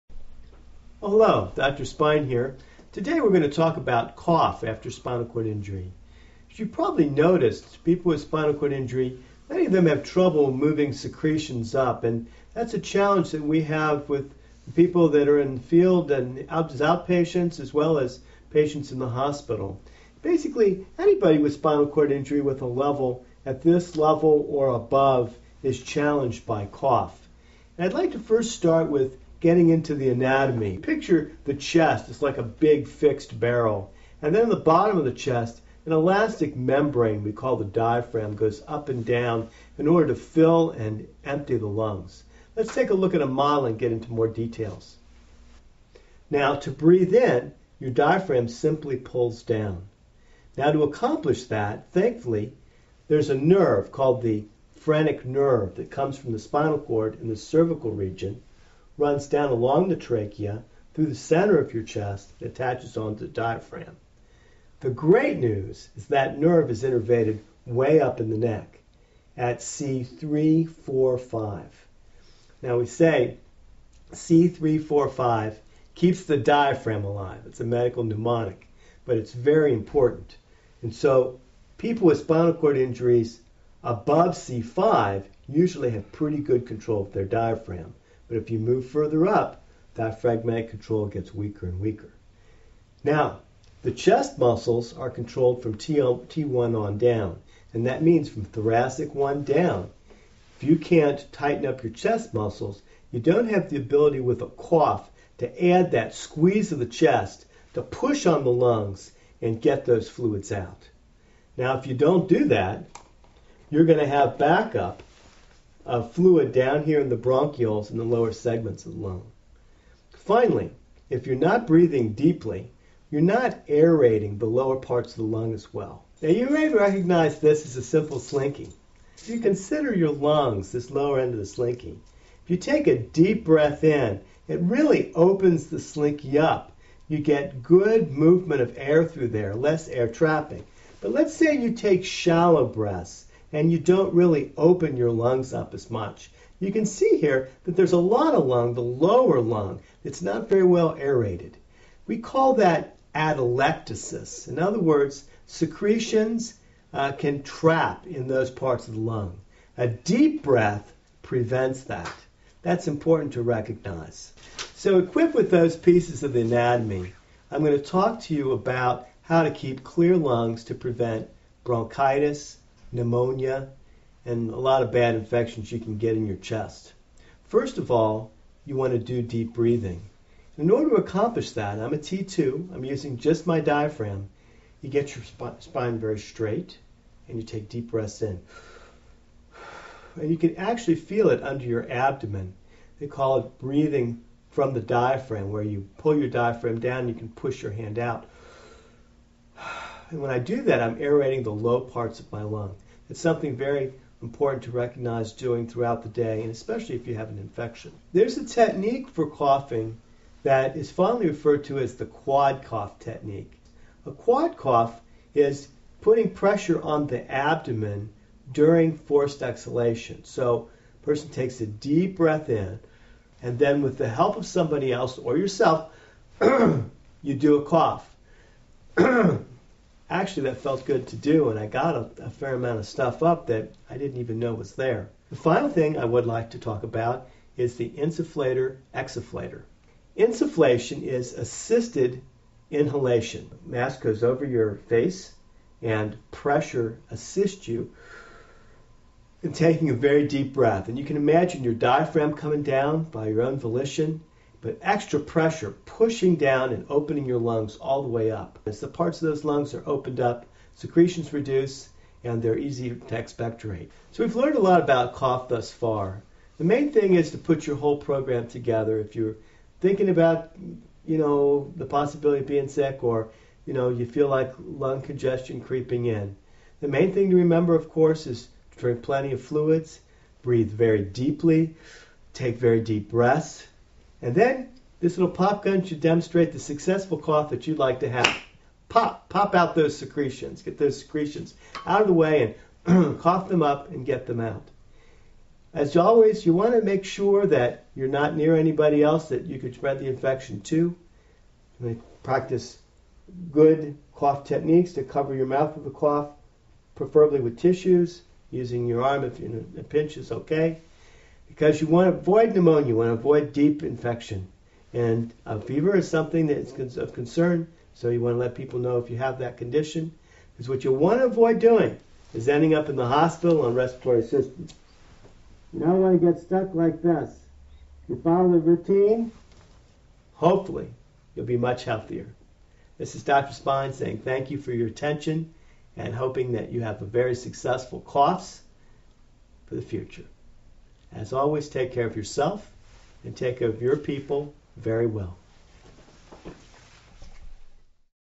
Dr. Spine - Cough
Dr. Spine explains the importance of coughing for the health of paraplegics and even more importantly for quads.